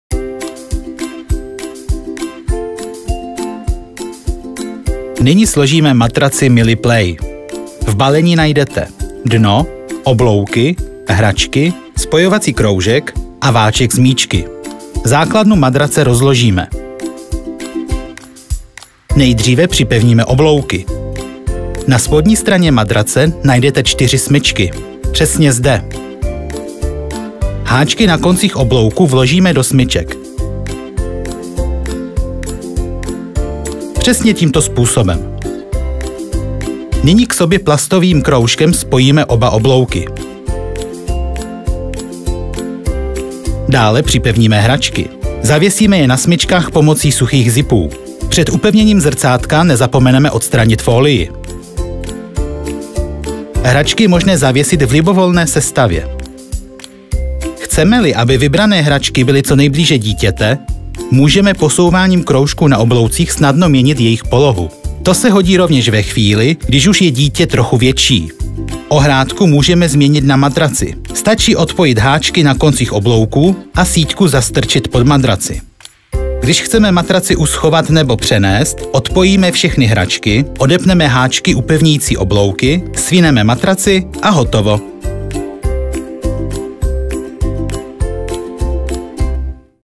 Vzdělávací podložka s míčky MILYPLAY Kinderkraft | Uživatelský manuál
Podívejte se, jak správně sestavit interaktivní vzdělávací podložku MILYPLAY od Kinderkraft! Ve videu se dozvíte, jak zvednout boky podložky a vytvořit suchý bazén, do kterého můžete dat barevné míčky, které jsou součásti sady. Ukážeme Vám také, jak nastavit čelenky tak, aby závěsné hračky byly blíže k dítěti. Podívejte se, jak snadno využívat všechny funkce a výhody vzdělávací podložky!